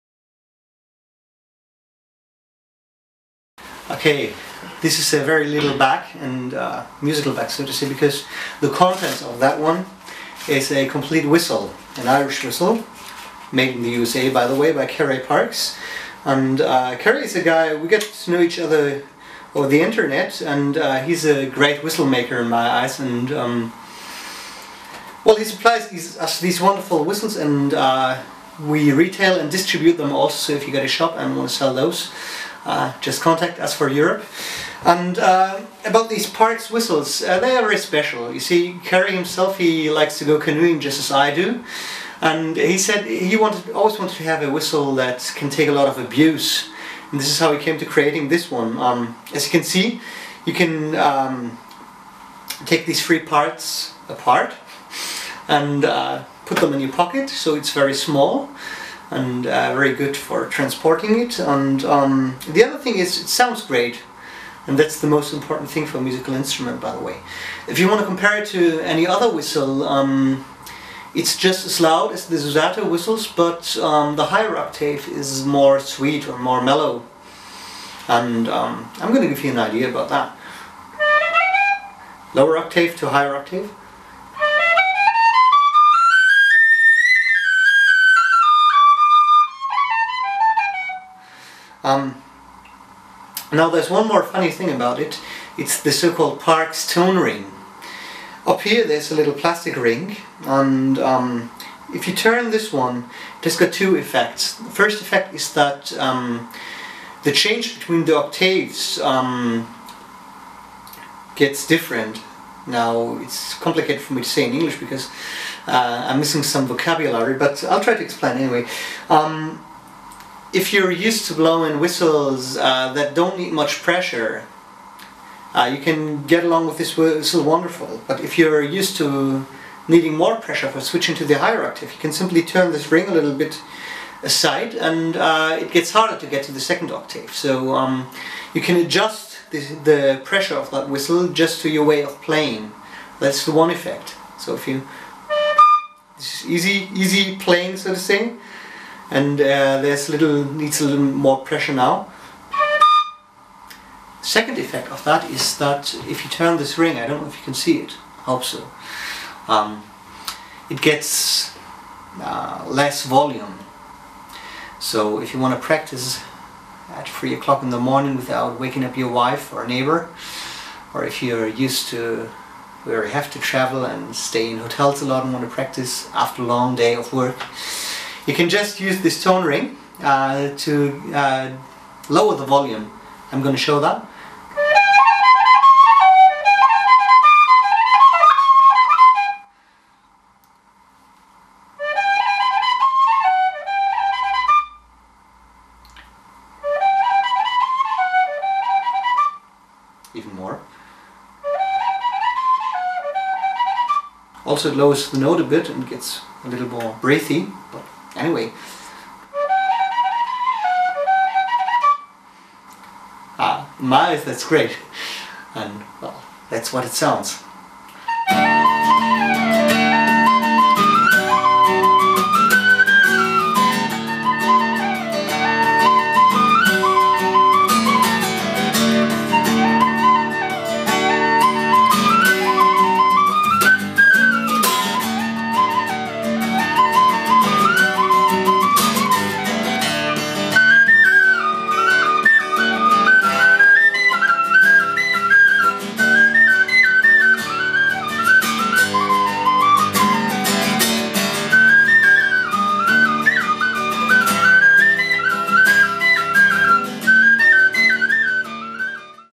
Parks walkabout travel tin whistle w. Parks tone ring
Heres an introduction to one of the most interesting whistles ever I saw. Take some minutes, get yourself a coffee, shut the door and lock out your cat and enjoy .